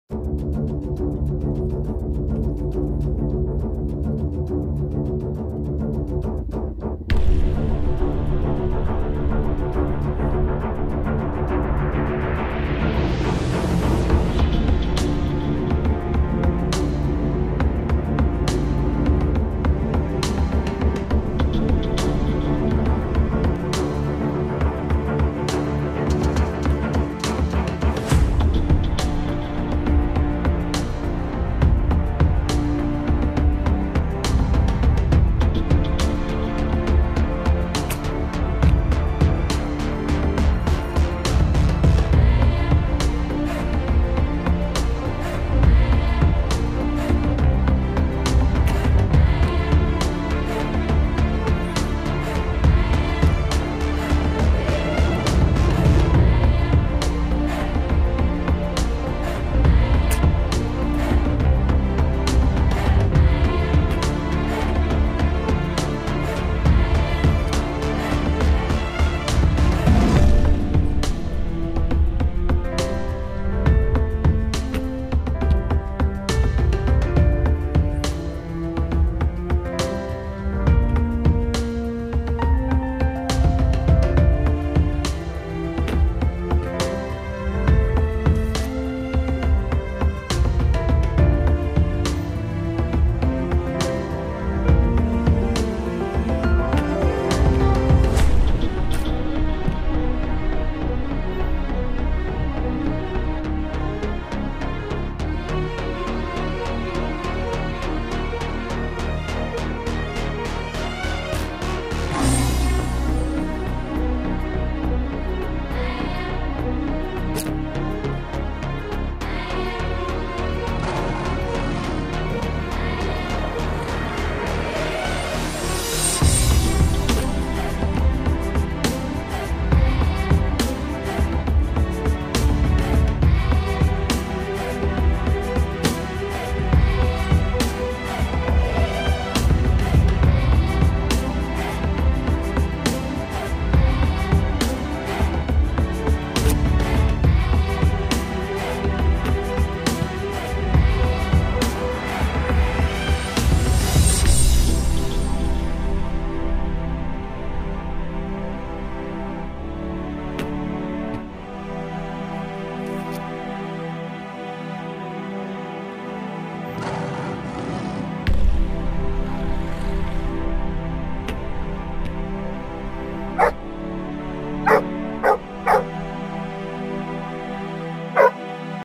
2023 03 19 14 04 20 주말에 시골에서 감성을 느끼며 베어보우쏘기~
주말에 시골에서 베어보우을 쏘며 감성을 느껴지며
힐링되내요~~